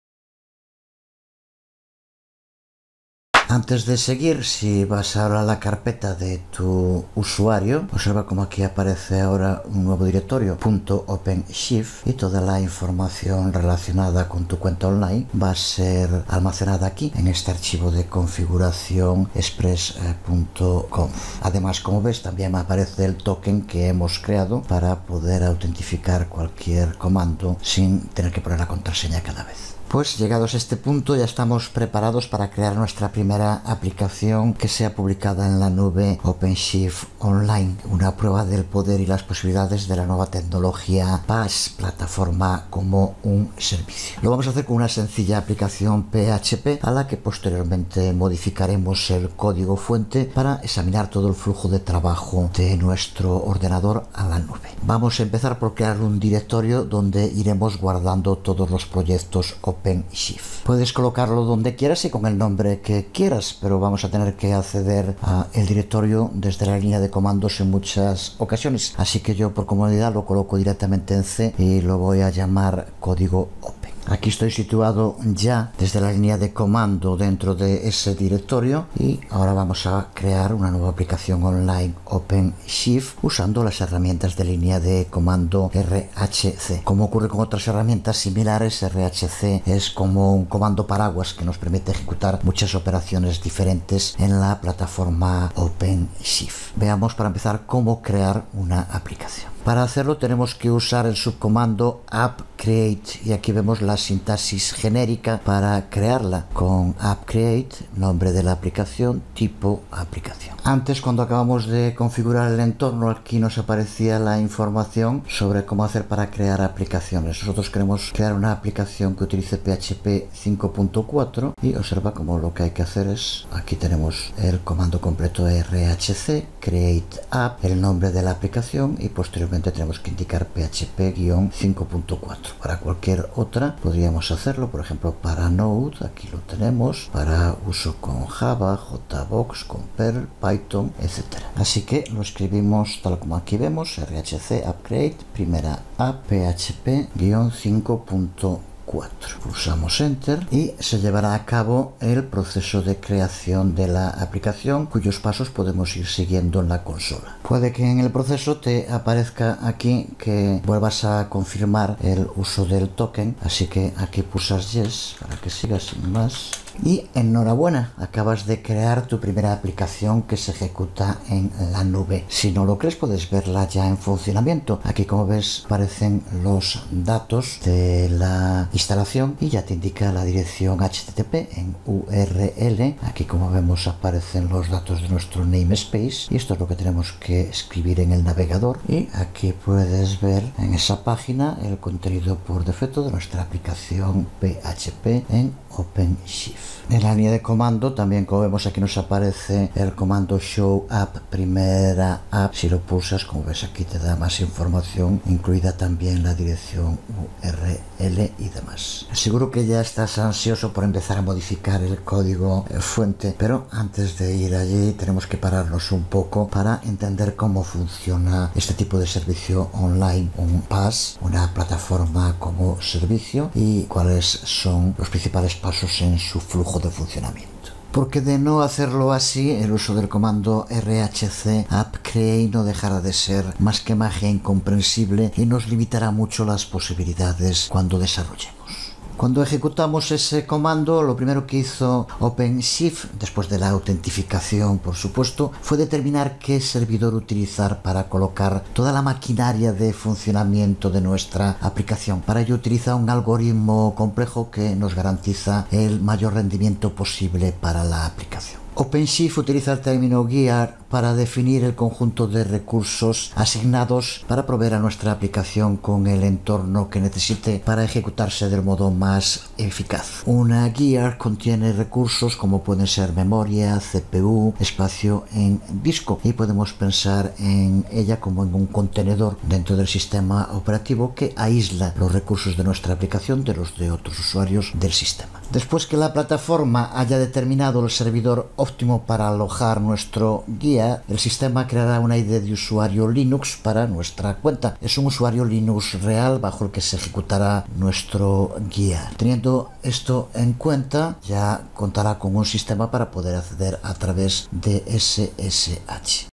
03.- Curso de OpenShift. Crear primera aplicación online
Videotutorial Nº 03 del Curso de OpenShift. Crear primera aplicación online. La carpeta .openshift en local; Crear aplicación con PAAS; Crear directorio local; Sintaxis rhc para crear una aplicación; rhc create-app; Crear primera aplicación; URL de acceso a la aplicación; Usar rhc show-app; Funcionamiento interno de Openshift; Lo primero determinar que servidor utilizar; Significado del Término GEAR; Creación ID Usuario Linux. 5 minutos.